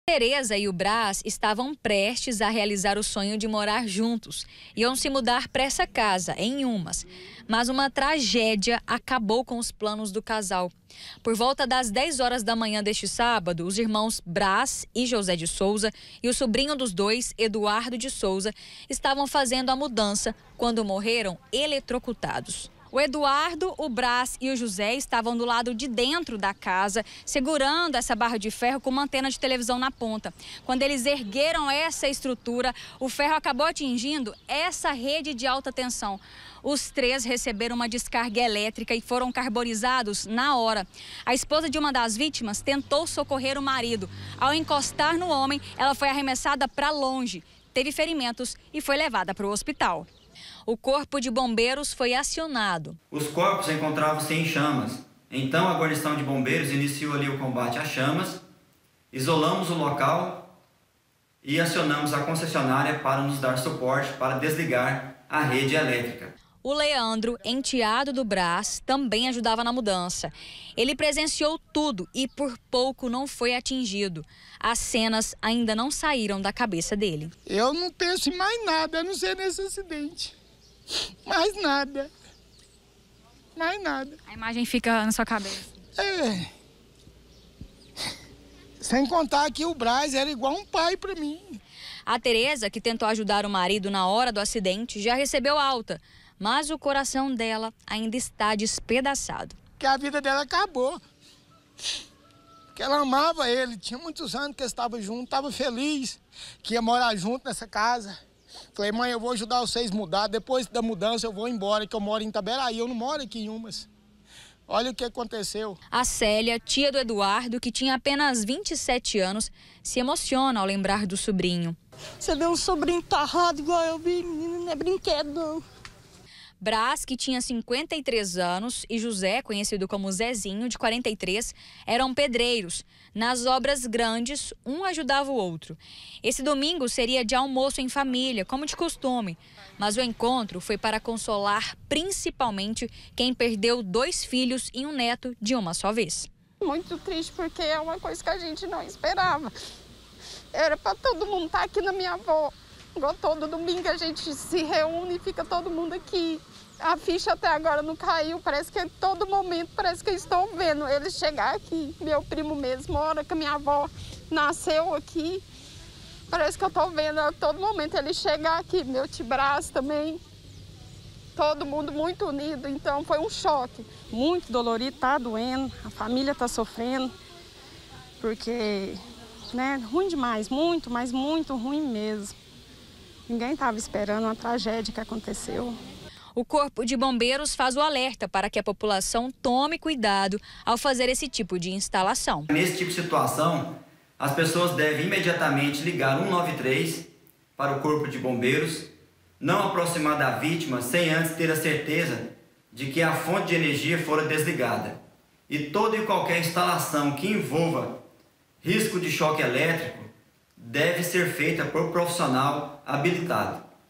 TRÊS PESSOAS MORREM APÓS CHOQUE EM INHUMAS
TRÊS PESSOAS  MORRERAM ELETROCUTADOS ENQUANTO INSTALAVAM UMA ANTENA DE TV EM UMA CASA NA VILA AMÉRICA, EM INHUMAS. AS VÍTIMAS FICARAM CARBONIZADAS.